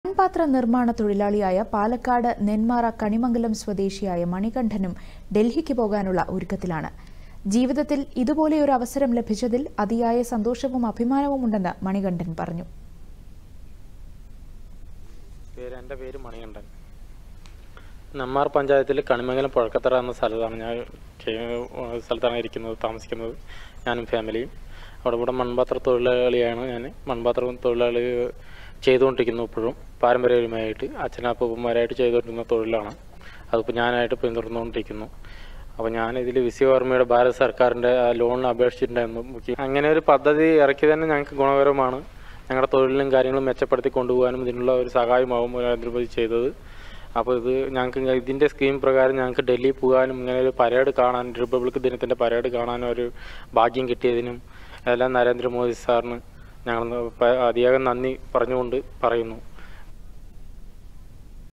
മൺപാത്ര നിർമ്മാണ തൊഴിലാളിയായ മണികണ്ഠൻ റിപ്പബ്ലിക്ക് ദിന പരേഡ് കാണാനുള്ള ഒരുക്കത്തിലാണ്
മൺപാത്ര നിർമ്മാണ തൊഴിലാളിയായ പാലക്കാട് നെന്മാറ കണിമംഗലം സ്വദേശിയായ മണികണ്ഠനും ഡൽഹിക്ക് പോകാനുള്ള ഒരുക്കത്തിലാണ്.

ജീവിതത്തിൽ ഇതുപോലെ ഒരു അവസരം ലഭിച്ചതിൽ അതിയായ സന്തോഷവും അഭിമാനവും ഉണ്ടെന്ന് മണികണ്ഠൻ പറഞ്ഞു.